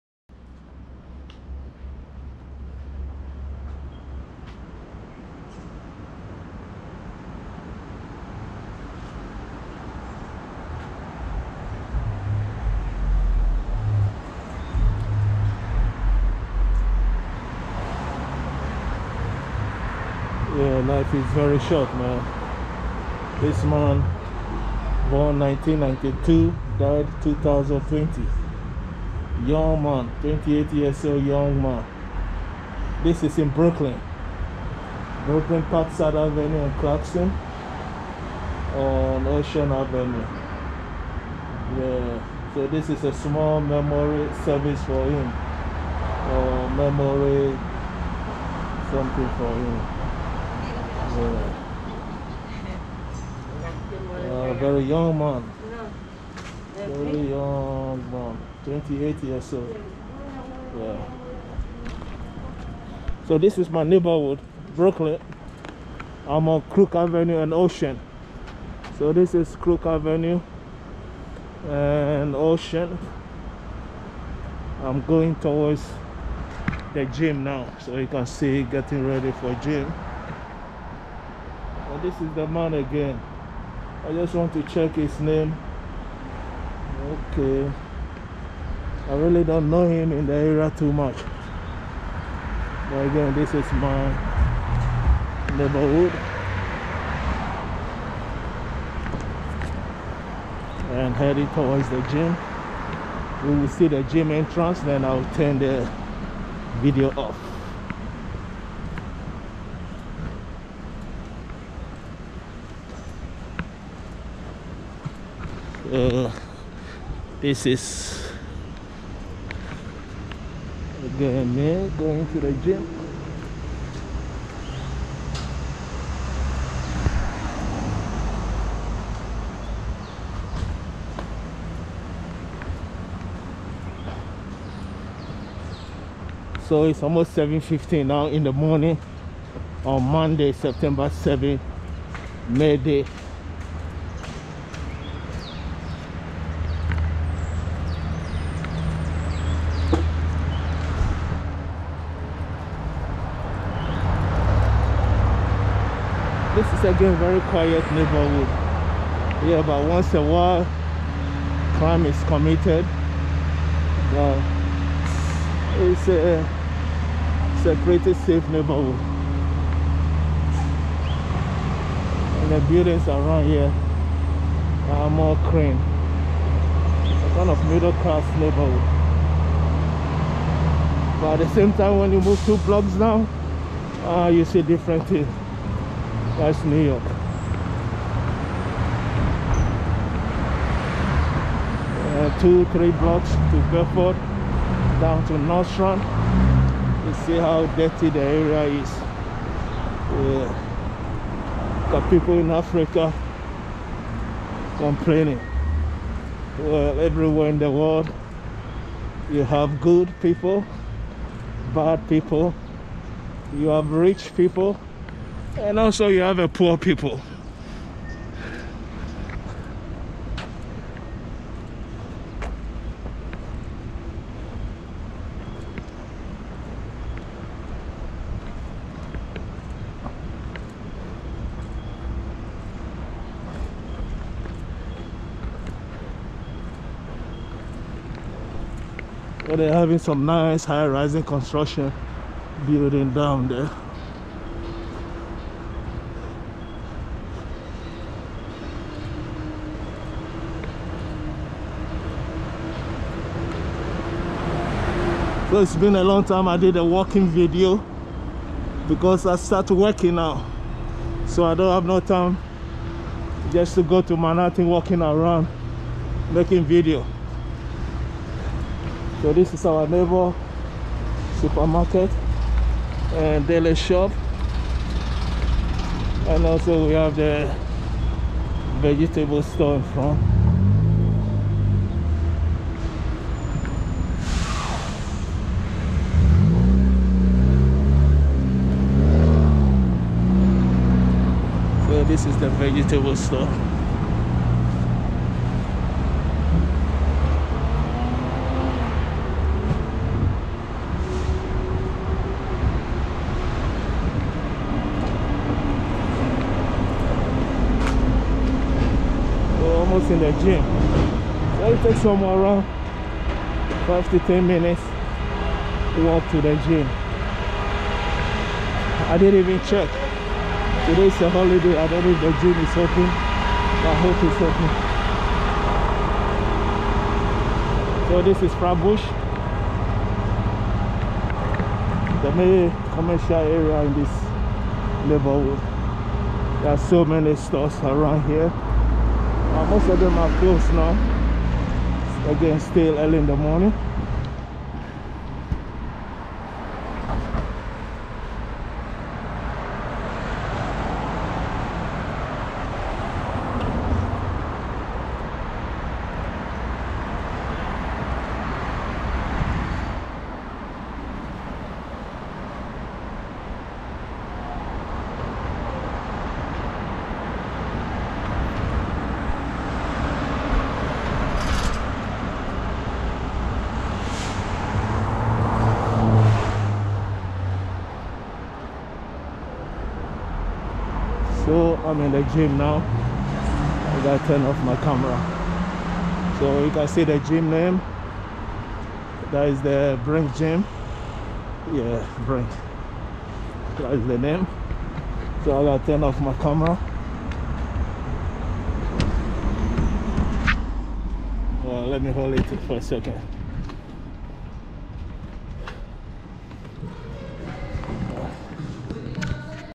Saw a Memorial Shrine for 28 years old man while going to Blink Fitness gym in Brooklyn
While going to the Blink Fitness gym at Flatbush, Brooklyn, I saw a Memorial Shrine for 28 years old man died in Brooklyn. May his soul RIP.